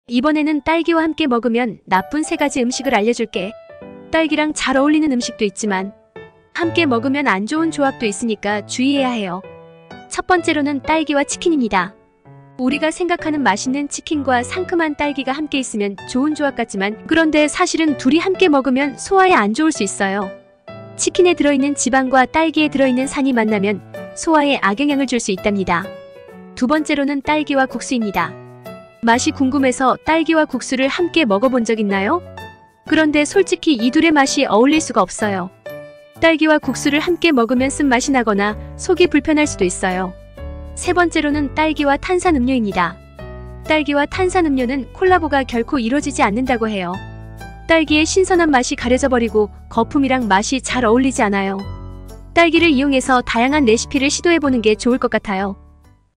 [음식궁합] • 딸기와 함께 먹으면 나쁜 3가지 음식『06』
[음식궁합] • 오렌지와 함께 먹으면 나쁜 3가지 음식

이번에는 딸기와 함께 먹으면 나쁜 3가지 음식을 알려줄게요! 딸기랑 잘 어울리는 음식도 있지만 함께 먹으면 안 좋은 조합도 있으니까 주의해야해요.

첫번째로는 딸기와 치킨입니다. 우리가 생각하는 맛있는 치킨과 상큼한 딸기가 함께 있으면 좋은 조합이겠지요? 그런데 사실은 둘이 함께 먹으면 소화에 안 좋을 수 있어요. 치킨에 들어있는 지방과 딸기에 들어있는 산이 만나면 소화에 악영향을 줄 수 있단 말이에요.

두번째로는 딸기와 국수입니다. 맛이 궁금해서 딸기와 국수를 함께 먹어본 적 있나요? 그런데 솔직히 이 둘의 맛이 어울릴리가 없어요. 딸기와 국수를 함께 먹으면 쓴 맛이 나거나 속이 불편할 수도 있어요.

세번째로는 딸기와 탄산음료야. 딸기와 탄산음료는 콜라보가 결코 이뤄지지 않는다고 해요. 딸기의 신선한 맛이 가려져버리고 거품이랑 맛이 잘 어울리지 않아요. 딸기를 이용해서 다양한 레시피를 시도해보는 게 좋을 것 같아요! 이런 신기한 조합들을 알아가는 재미도 있답니다.

채널을 찾는 구독자 여러분 모두에게 도움 되도록 항상 새로운 영상을
만들도록 노력하겠습니다!

copyright by : Miracle Healing (@tvsang624)